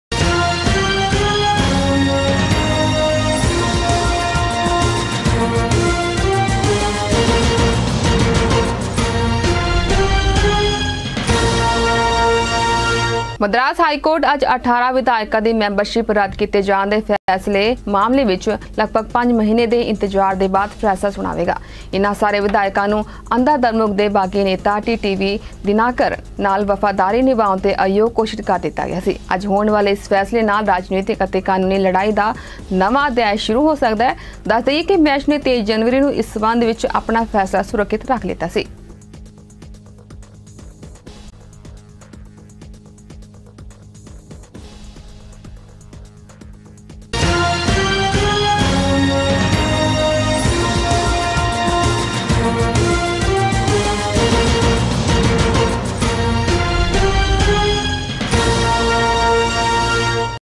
18 ਵਿਧਾਇਕਾਂ ਦੀ ਮੈਂਬਰਸ਼ਿਪ ਰੱਦ ਕੀਤੇ ਜਾਣ ਮਾਮਲੇ 'ਤੇ ਅੱਜ ਆਵੇਗਾ ਫੈਸਲਾ: ਮਦਰਾਸ ਹਾਈਕੋਰਟ
18 ਵਿਧਾਇਕਾਂ ਦੀ ਮੈਂਬਰਸ਼ਿਪ ਰੱਦ ਕੀਤੇ ਜਾਣ ਮਾਮਲੇ 'ਤੇ ਅੱਜ ਆਵੇਗਾ ਫੈਸਲਾ: ਮਦਰਾਸ ਹਾਈਕੋਰਟ